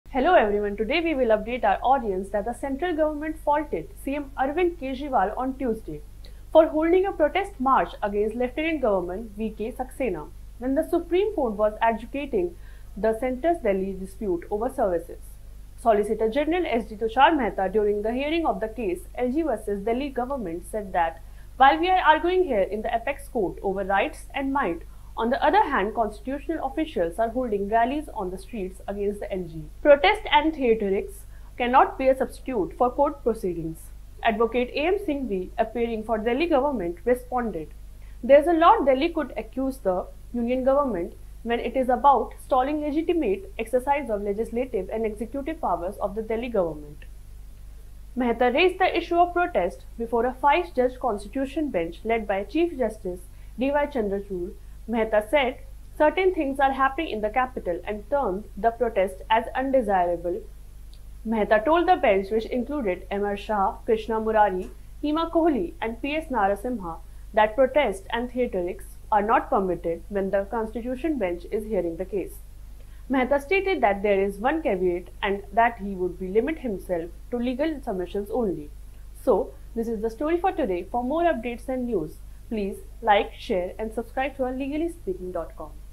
Solicitor General in SC: ‘Protest and Theatrics cannot be a substitute for court proceedings’
Central Government faulted CM Arvind Kejriwal on Tuesday for holding a protest march against LG V K Saxena when the Supreme Court was adjudicating the Centre-Delhi dispute over “services.”